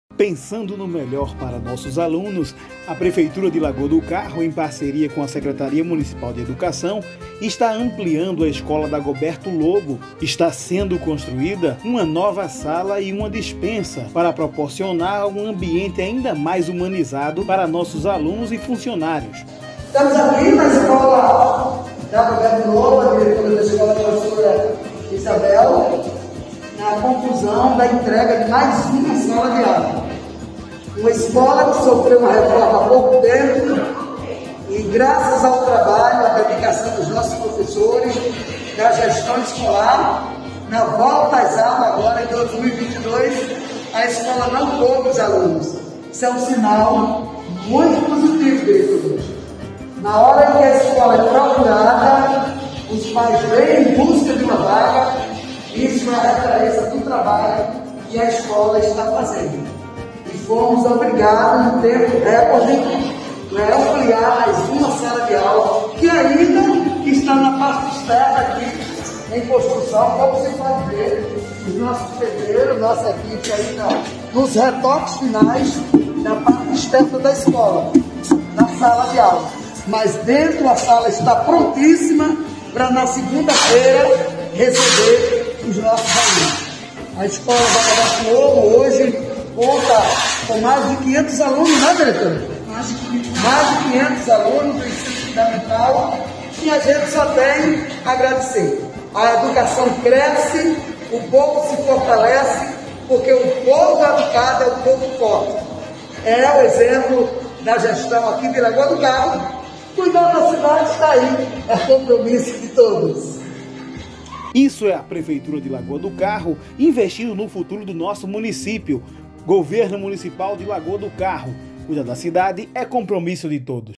Ampliação na Escola Dagoberto Lobo -2022
Sempre valorizando a Educação do nosso Município, a Prefeitura de Lagoa do Carro esta realizado uma ampliação na Escola Dagoberto Lobo.

Estão sendo construídas uma nova sala e uma despensa proporcionando uma ambiente melhor para os nossos alunos e equipe escolar!